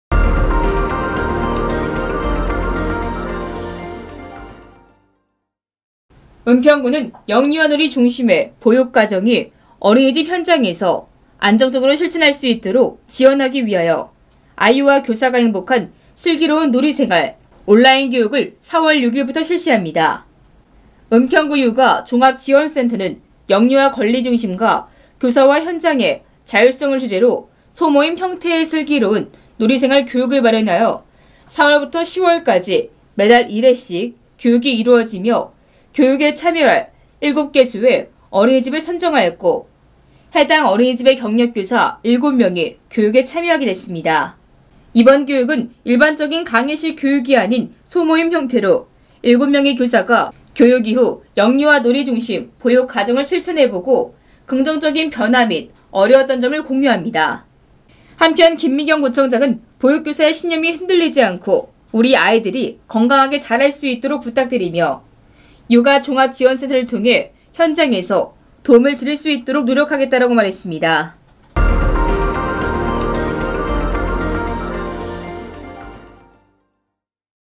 은평구 김미경구청장 「슬기로운 놀이생활」 온라인 교육 실시 은평구육아종합지원센터 국민의소리TV 서울포털TV
은평구(구청장 김미경)는 영유아 놀이 중심의 보육과정이 어린이집 현장에서 안정적으로 실천할 수 있도록 지원하기 위하여 아이와 교사가 행복한 「슬기로운 놀이생활」 온라인 교육을 4월 6일부터 실시한다.

은평구육아종합지원센터에는 영유아 권리 중심과 교사와 현장의 자율성을 주제로 소모임 형태의 슬기로운 놀이생활 교육을 마련하여 4월부터 10월까지 매달 1회씩 교육이 이루어지며 교육에 참여할 7개소 어린이집을 선정하였고 해당 어린이집의 경력교사 7명이 교육에 참여하게 된다.

이번 교육은 일반적인 강의식 교육이 아닌 소모임 형태로 7명의 교사가 교육 이후 영유아 놀이 중심 보육과정을 실천해보고 긍정적 변화 및 어려웠던 점 등을 공유하여 교사 주도적 활동에서 영유아의 흥미와 요구가 반영된 놀이가 이루어지는 어린이집으로 변화할 수 있도록 진행될 예정이다.

김미경 은평구청장은 “보육교사의 신념이 흔들리지 않고 우리 아이들이 건강하게 자랄 수 있도록 부탁드리며, 육아종합지원센터를 통하여 현장에서 도움을 드릴 수 있도록 노력하겠다.”고 밝혔다.